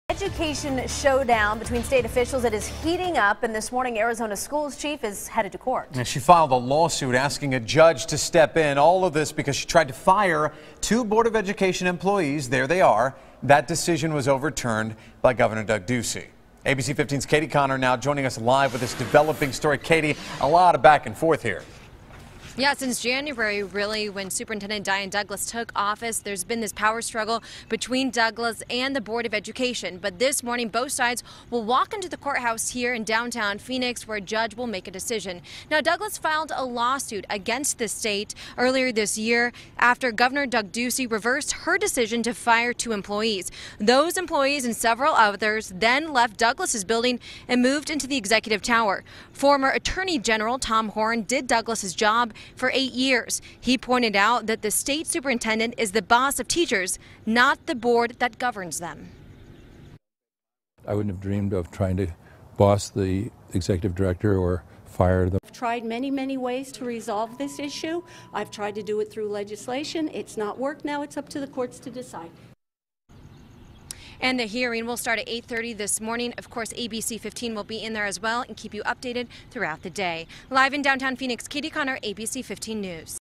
Diane Douglas heads to court
The Board of Education and State Superintendent will face off on Thursday.